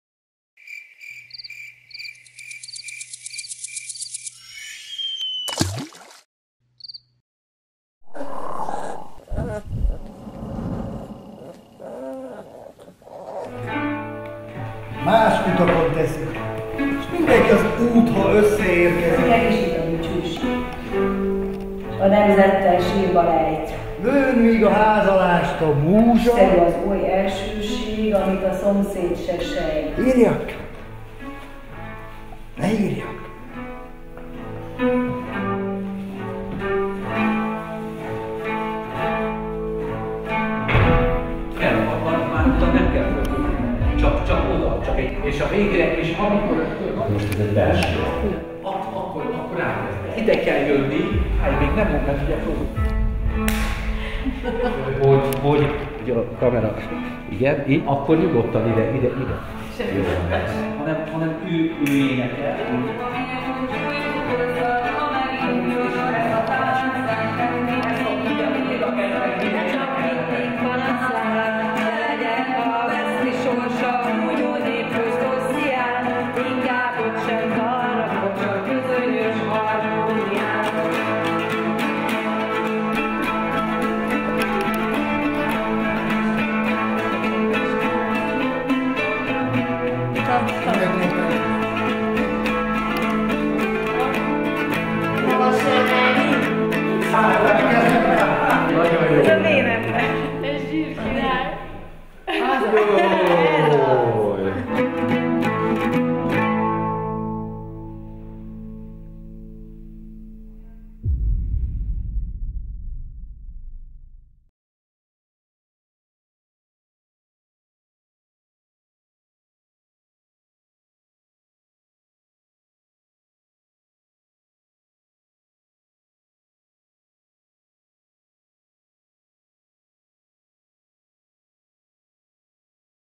Az Igric - a Medve Színpad előadása - a száraz próba "beszédes" részlete
Arany János élete, titka.
A költészet rítusa, a rítus költészete.
Az Almássy Produkció és a Medve Színpad előadása.

AZ IGRIC című előadás alapja Oláh Miklós Hungária és Attila című művei, mint Arany János hun trilógiájának forrásai, Szörényi László "De mi híja látszik a mélylő tengernek"- Arany János születésének 200. évfordulója elé, valamint Szilágyi Márton „van ott sokféle faj” című tanulmánya.

Szereplők: Apró Anna, Gecse Noémi, Olasz Etelka, Berényi Dávid, Válóczy Elek
Táncosok, koreográfus: Sándor Tímea, Soós Viktória
Zenei közreműködő, zeneszerző: Apró Anna (kobzos), Buda Ádám (kobzos)
Látványtervező: Velich Dóra Teo
Forgatókönyvíró: Weiner Sennyey Tibor, Halmy György
Szakértők: dr. Szörényi László irodalomtörténész, Rossa László zeneszerző
Rendező: Halmy György